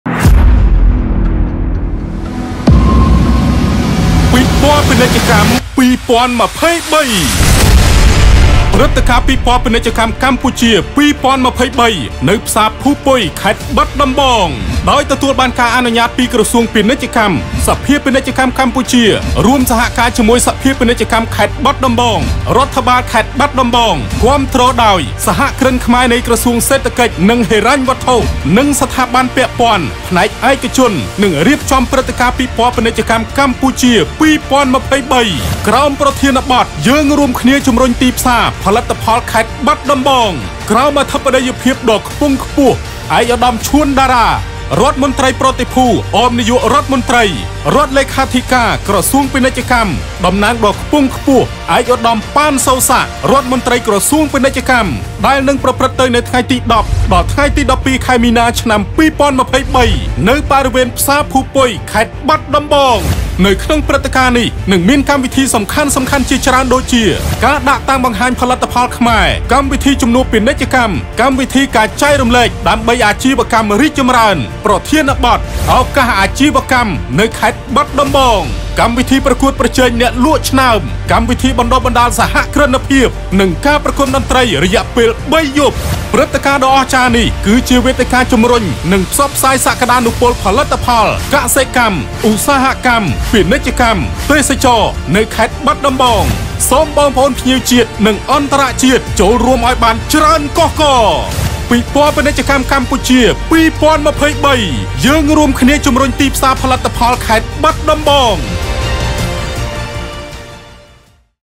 BUSINESS EXHIBITION PROMOTE #01 | ពិព័រណ៍ពាណិជ្ជកម្មកម្ពុជា ២០២៣
ព្រឹត្តិការណ៍ ពិព័រណ៍ពាណិជ្ជកម្មកម្ពុជា ២០២៣ ក្រោមប្រធានបទ យើងរួមគ្នាជំរុញទីផ្សារ ផលិតផលខេត្តបាត់ដំបង រៀបចំដោយ សភាពាណិជ្ជកម្មកម្ពុជា រួមសហការជាមួយ សភាពាណិជ្ជកម្មខេត្តបាត់ដំបង រដ្ឋបាលខេត្តបាត់ដំបង គាំទ្រដោយ ក្រសួងពាណិជ្ជកម្ម សហគ្រិនខ្មែរ នៃក្រសួងសេដ្ឋកិច្ចនិងហិរញ្ញវត្ថុ និងស្ថាប័ន ពាក់ព័ន្ធ និង ផ្នែកឯកជន។

ព្រឹត្តិការណ៍នេះ នឹងមានកម្មវិធីសំខាន់ៗជាច្រើនដូចជា៖ ការដាក់តាំងបង្ហាញផលិតផលខ្មែរ កម្មវិធីជំនួបពាណិជ្ជកម្ម កម្មវិធីការចែករំលែកដើម្បីអាជីវកម្មរីកចម្រើន ប្រធានបទ«ឱកាសអាជីវកម្មនៅខេត្តបាត់ដំបង» កម្មវិធីប្រកួតប្រជែងប្រជែងអ្នកលក់ឆ្នើម កម្មវិធីបណ្តុះបណ្តាលសហគ្រិនភាព និងការប្រគុំតន្រ្តីជាដើម។